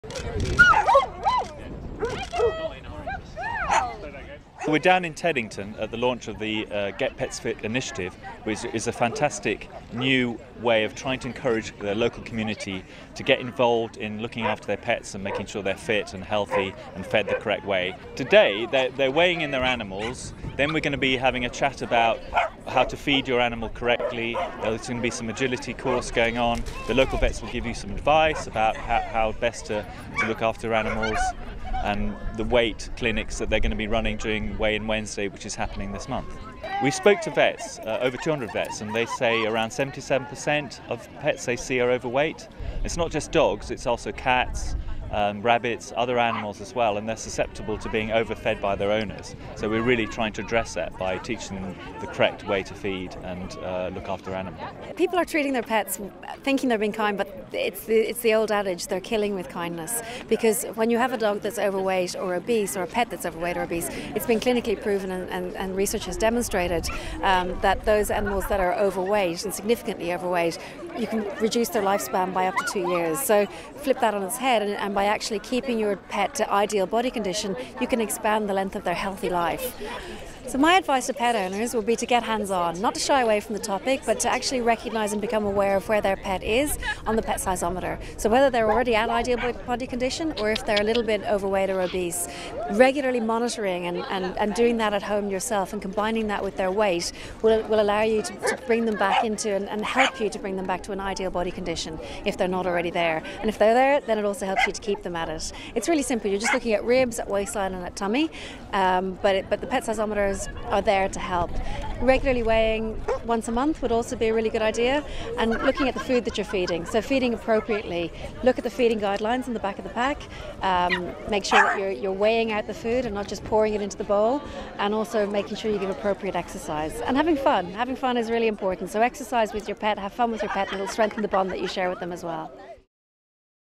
How to get your pets fit
Does your dog get out of breath on those long beach walks? Does it prefer lounging on the sofa than playing with it's friends? Get Pets Fit launched in Teddington to get our pets back on track with fitness and diets for a healthy life.